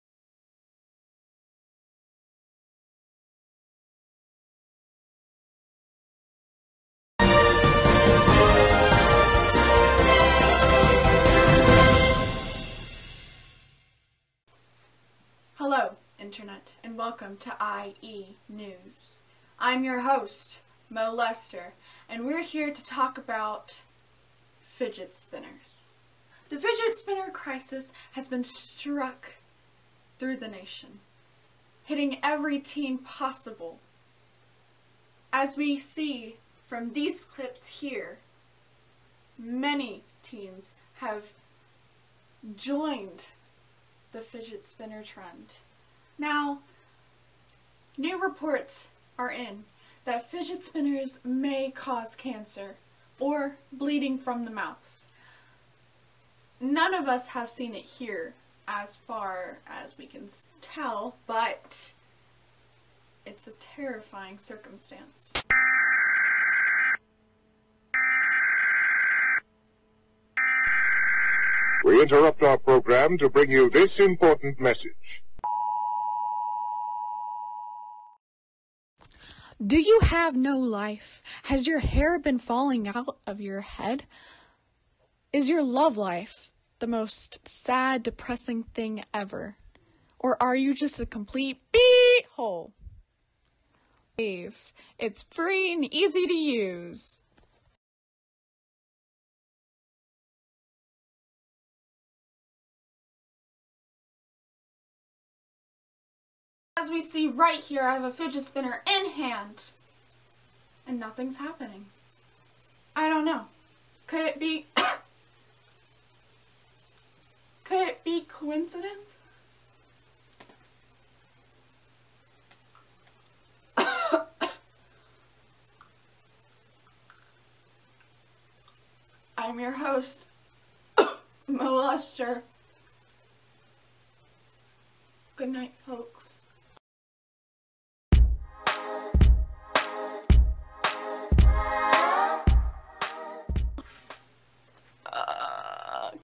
Internet Explorer News (Fidget Spinners)
This was kind of a random idea of mine,  since internet explorer is so slow I thought about "what if it was a news channel", hence covering the old topic of fidget spinners.
It's dumb, but I thoroughly enjoyed making this.